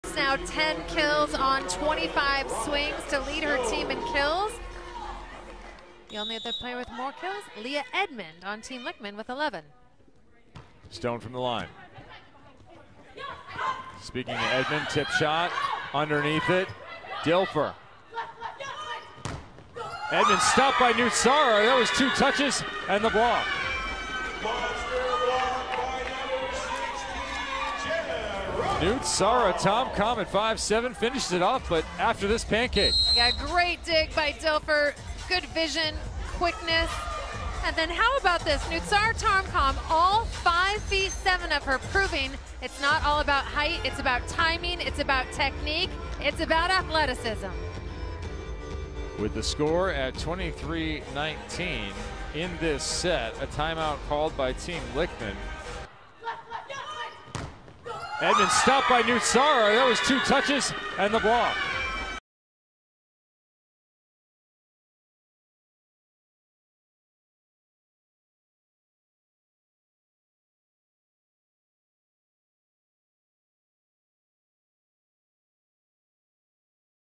ซาร่า นุศรา ต้อมคำ จังหวะขึ้นบล็อกลูกตบของ Edmond | Highlight | AUVB SS2
Thank you original video clip from Youtube & Facebook : Athletes Unlimited

ขอบคุณที่สนับสนุนและติดตามกันนะครับ 🙏🏻